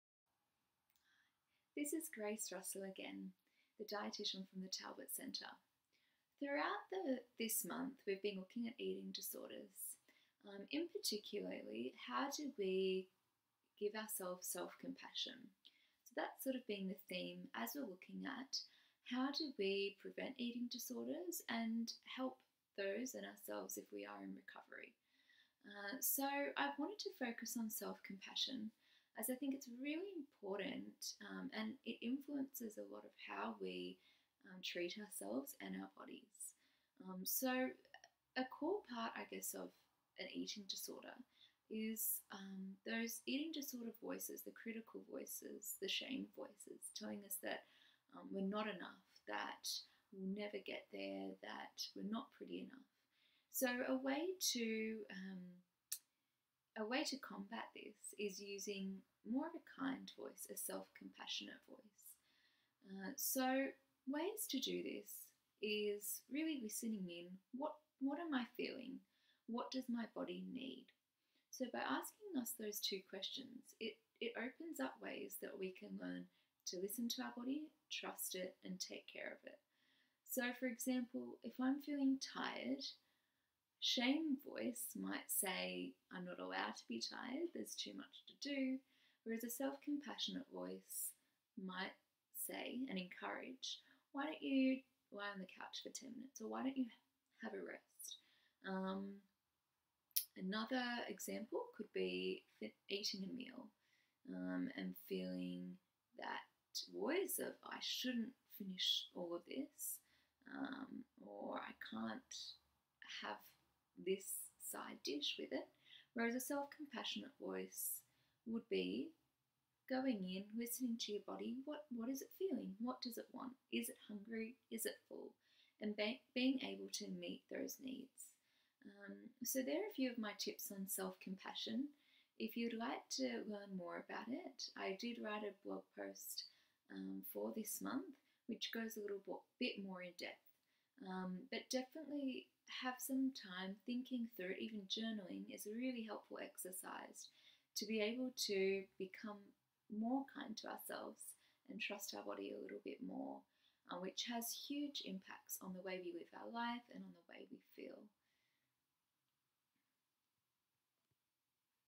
Self-Compassion with Grace Russell
The Talbot Centre dietitian, Grace Russell, speaks about self-compassion.